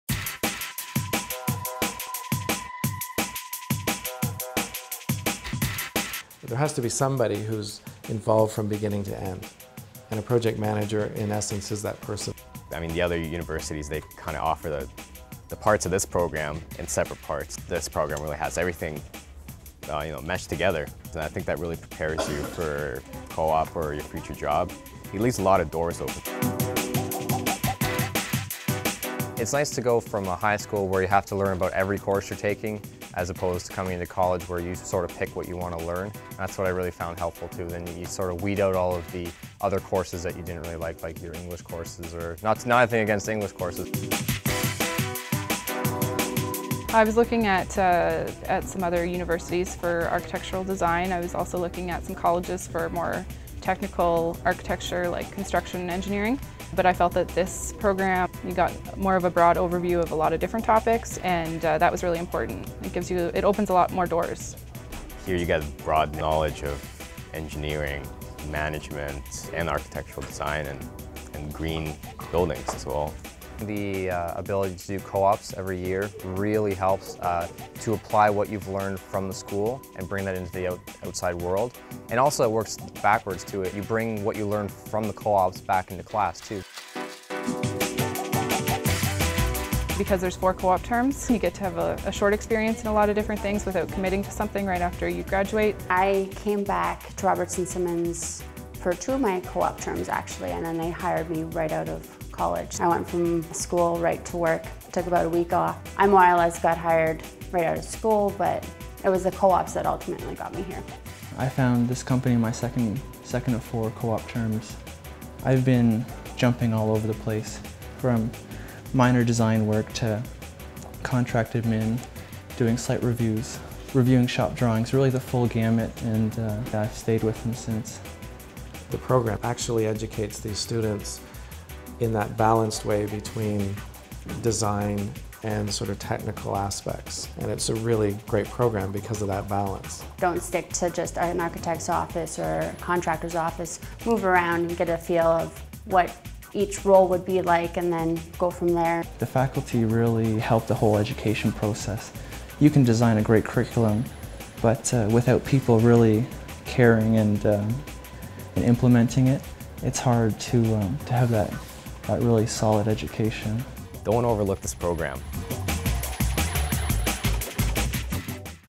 Architecture -- Project and Facility Management
Graduates of this four-year, co-op program will be educated and trained specifically to supervise and manage projects and facilities for the built environment. Students will graduate with strong foundations in architecture, construction, basic engineering and business, comprehensive insight into facility management and exposure to both liberal arts disciplines and practical applications for the business enterprise. All dimensions will involve creativity, leadership training, teamwork, and individual initiative, emphasizing the practical application of classroom studies. This degree program is recognized by the International Facility Management Association (IFMA) and graduates are eligible to become Certified Facility Managers after 3 years of related work experience and writing the CFM exam.